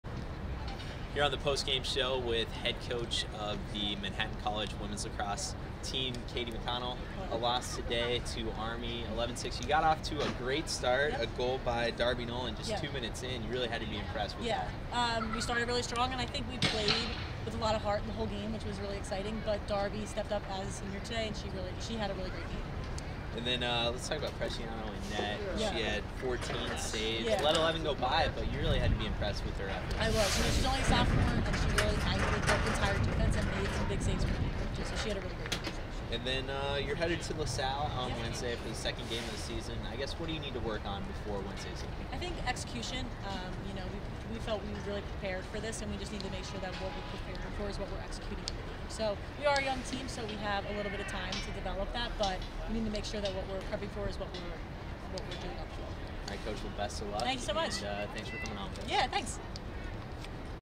JSN - Manhattan WLAX Postgame (2.18.17)
Head coach Katie McConnell joins the Jasper Sports Network (JSN) to discuss Manhattan's 11-6 loss to Army.